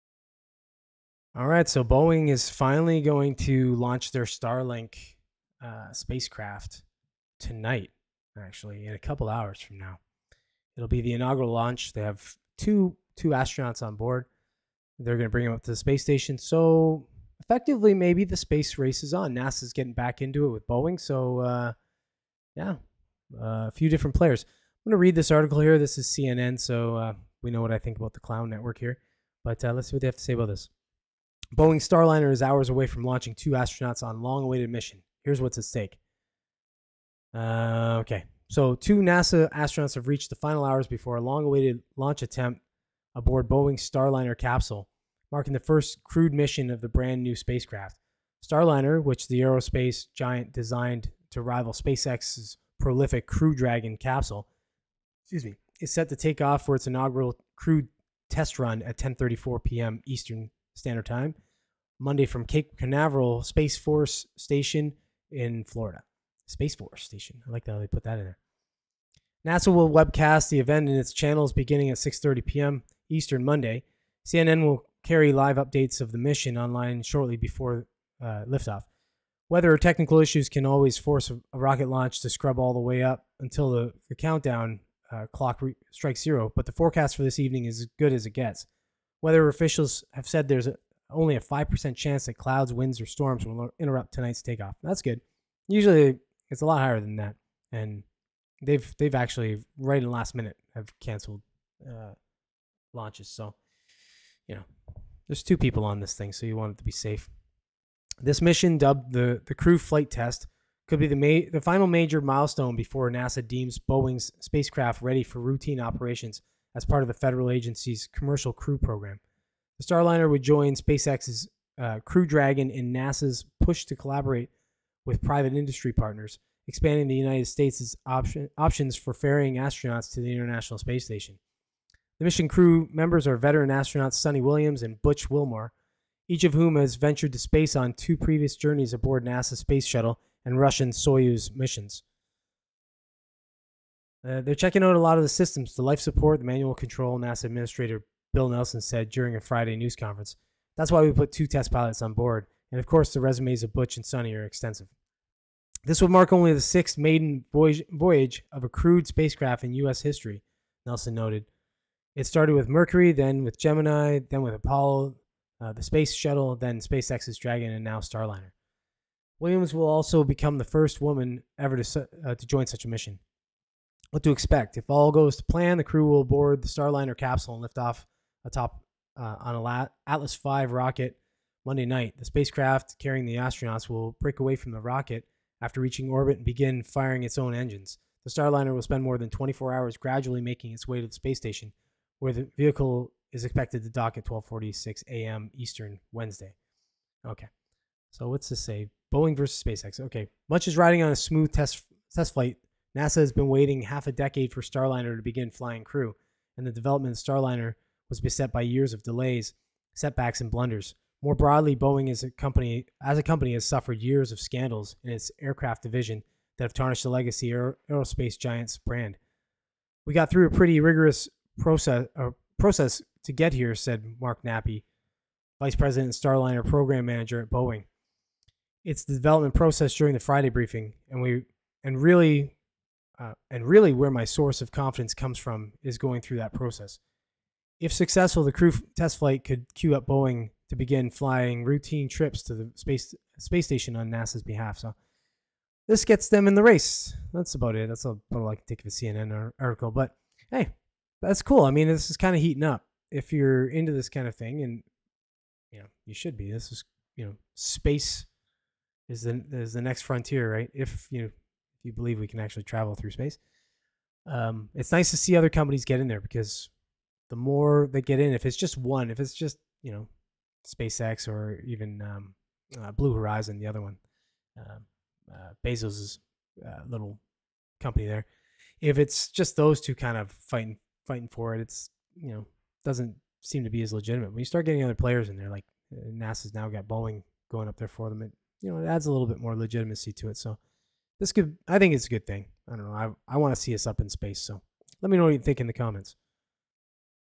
Boeing's Starliner Launch - Nasa re-enters space race
In this clip from my weekly podcast I talk about the recent launch of Boeing's Starliner Spacecraft.  This marks Nasa's official entry into the space race joining the likes of Musk's Space-X and Bezo's Blue Horizon.  Will this heat this race up and push us further toward actual space travel?